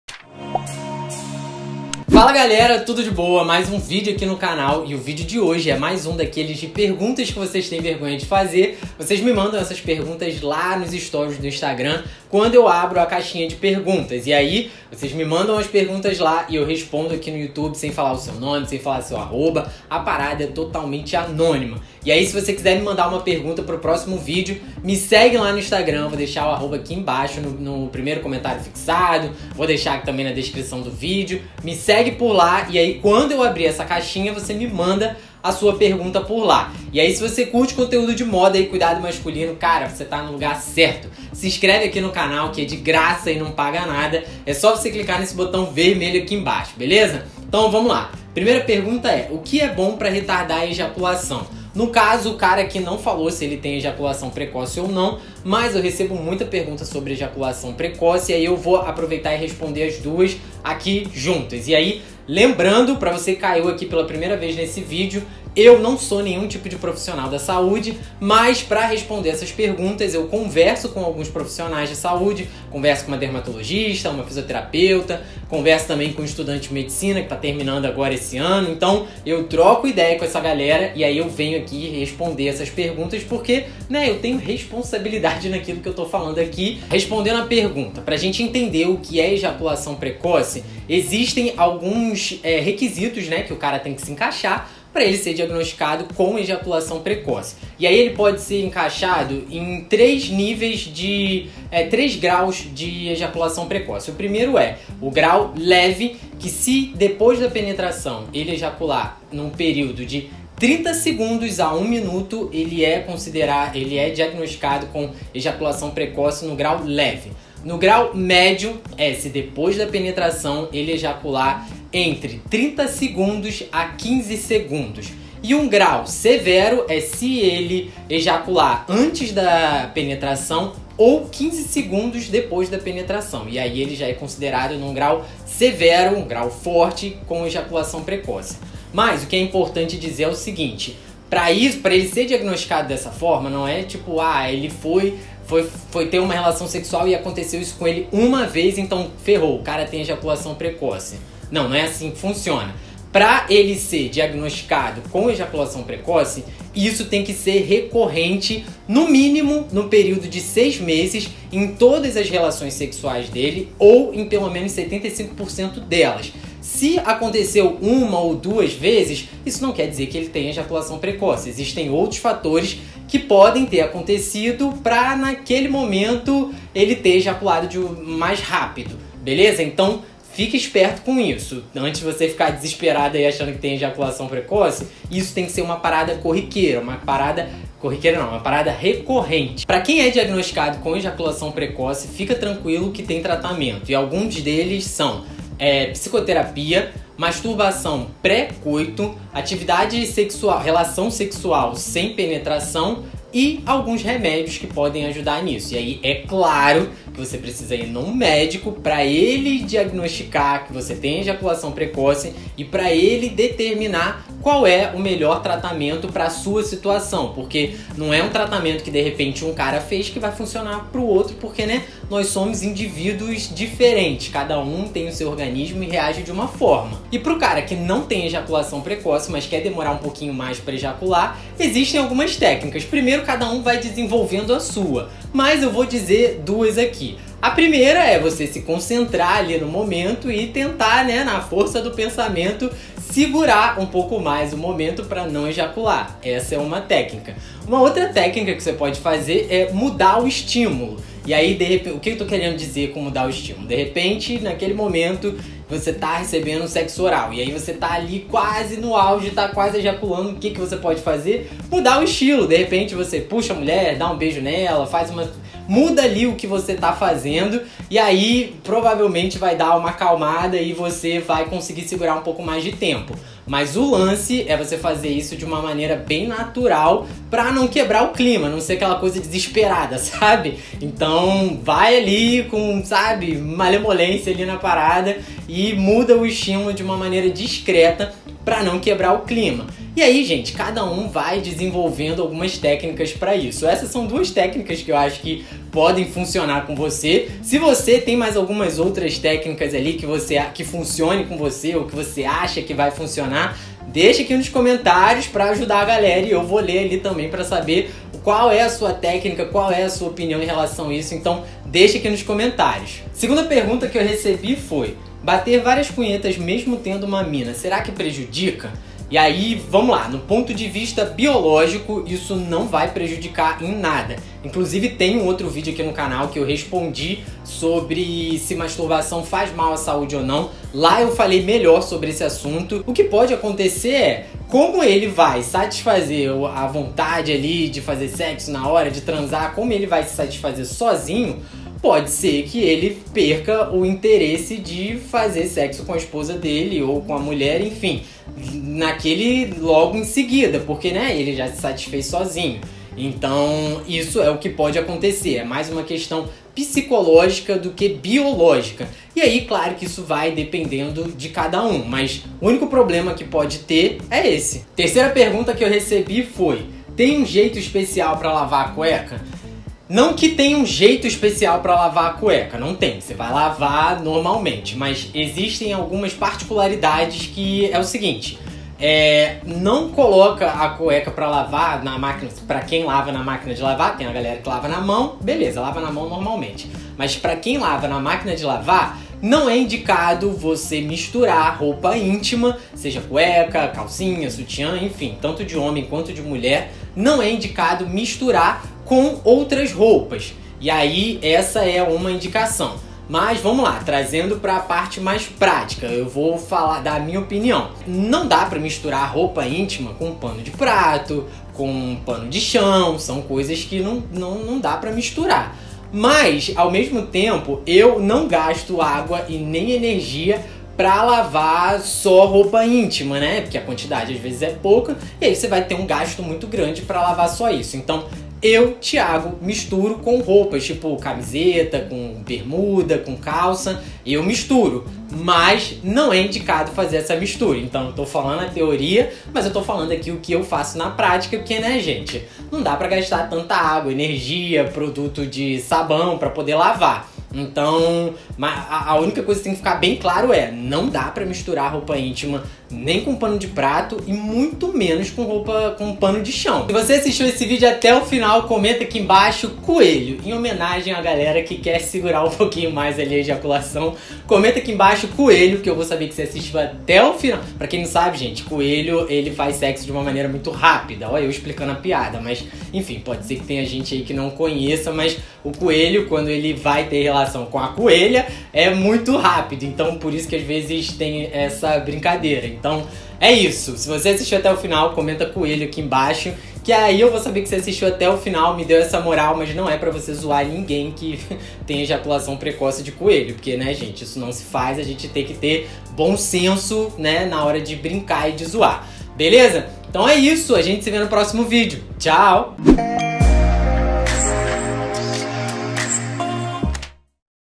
Como demorar a ejacular? Faz mal se masturbar tendo namorada?
Existem algumas perguntas que alguns homens tem vergonha de fazer. Uns por conta da masculinidade frágil, e outros por medo de serem perguntas bobas.
Por isso, eu resolvi juntar  algumas dessas perguntas, e responder nesse vídeo. Algumas dessas dúvidas são tão comuns, que pode ser que você também tenha.